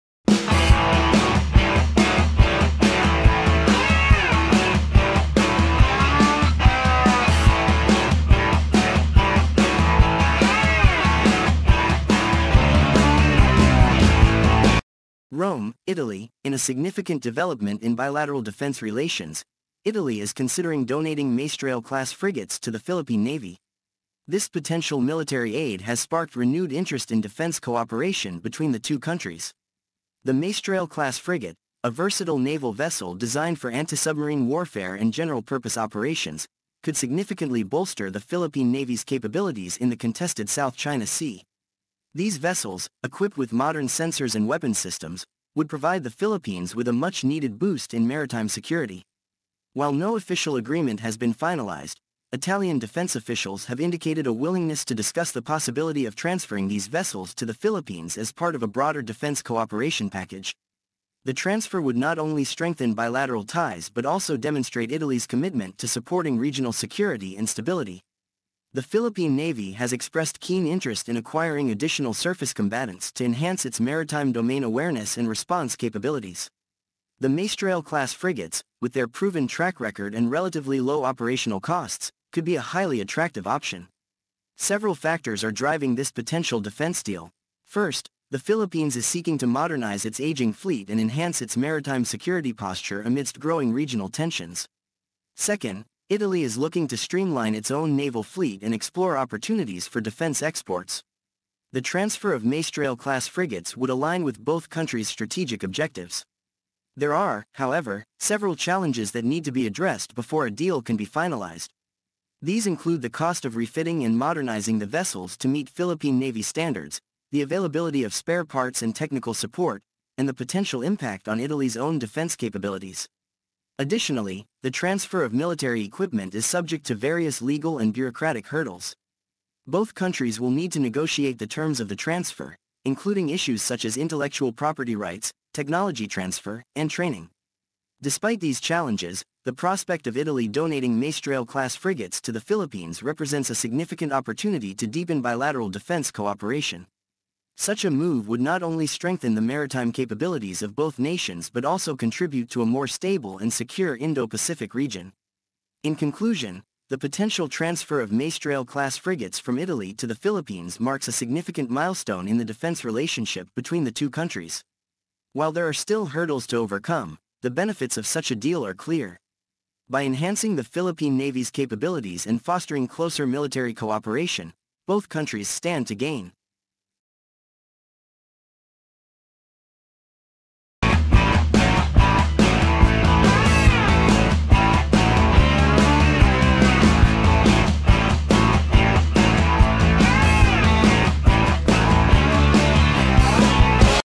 Italy Weighs Donating Maestrale-Class Frigates to the Philippines
Philippines and Italy Seek to Strengthen Defense TiesRome, Italy – The Philippines and Italy are poised to deepen their defense cooperation, with both countries aiming to finalize a new Memorandum of Understanding (MoU) by 2025.Philippine Ambassador to Italy Neal Imperial confirmed the development during a courtesy call with Italian Defense Minister Guido Crosetto. The new MoU aims to enhance defense relations through joint training, military education, and potential partnerships in defense industry development and technology transfer.The move comes after a period of relative inactivity in defense cooperation between the two countries. Previous efforts, such as the acquisition of refurbished Maestrale-class frigates during the Aquino administration, were ultimately canceled. However, recent diplomatic initiatives have led to a renewed interest in strengthening defense ties.The Philippines and Italy share common security interests, particularly in maritime security and counterterrorism. The new MoU is expected to facilitate greater cooperation in these areas, including joint patrols, information sharing, and capacity building.By deepening their defense partnership, the Philippines and Italy can contribute to regional stability and security. This renewed engagement could also open up new opportunities for trade and investment between the two countries.